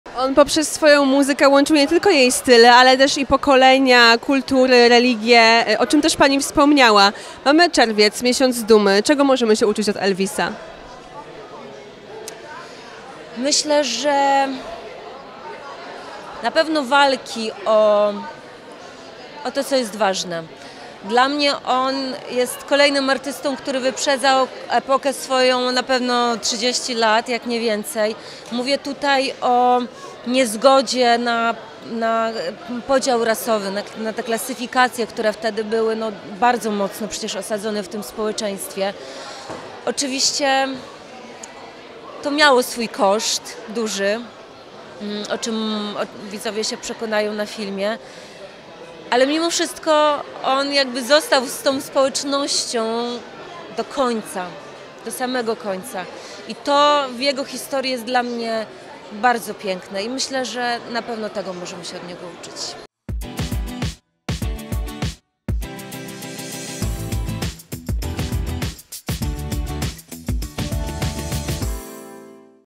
Co Elvis chciałby przekazać podczas PRIDE MONTH? O tym Katarzyna Zawadzka!
Gdyby Elvis mógł, na pewno zabrałby głos podczas miesiąca dumy. O tym jak on walczył z nierównościami klasowymi czy społecznymi opowiedziała Katarzyna Zawadzka.
Już od 24 czerwca w kinach będziecie mogli doświadczyć widowiska światowej klasy. Film "Elvis", z główną rolą Austina Butlera to niezapomniane połączenie wybitnych ruchów scenicznych, zapadających w pamięć strojów i wiecznie żywej muzyki. Całości dopełnia wielowymiarowa rola pułkownika Toma Parkera, grana przez wybitnego Toma Hanksa. Tego nie możecie przegapić!